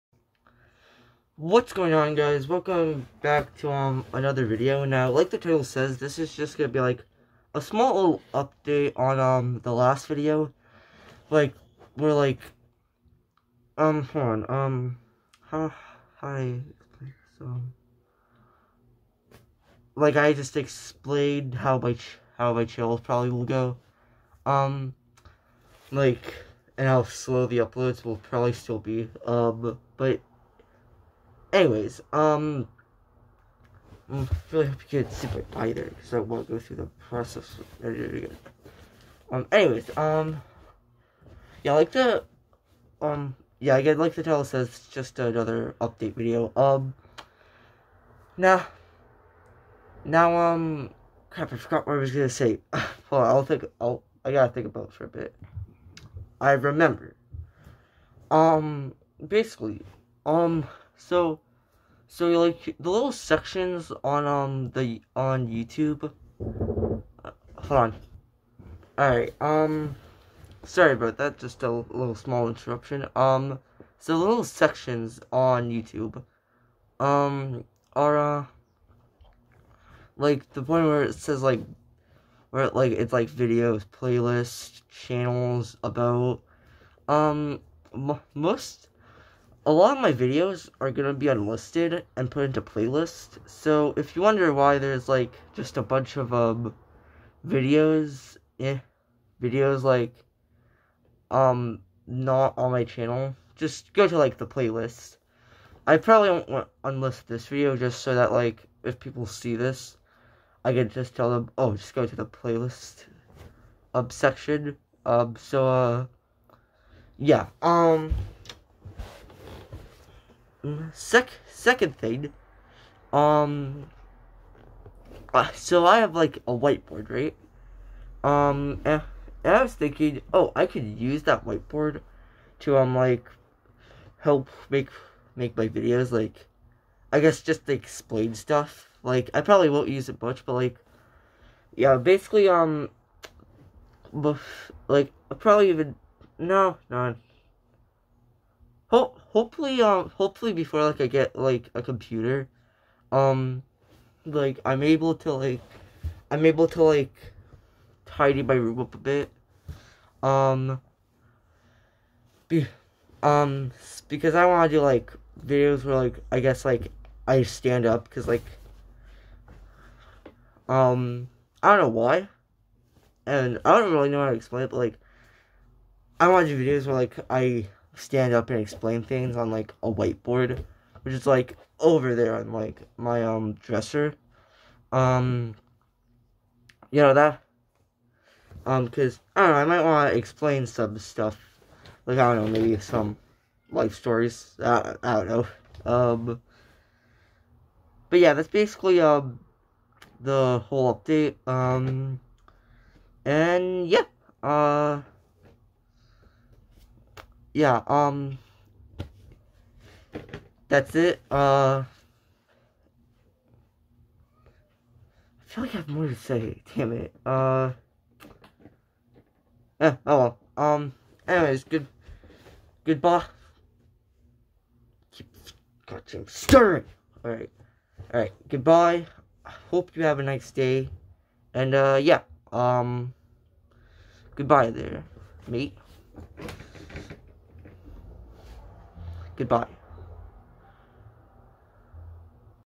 Small Second update for my Channel
You can slightly see my right eye through the glass lol, it doesn't really matter that much since it's kinda hard to see, and people probably won't be able to figure out my face because of it so I should be fine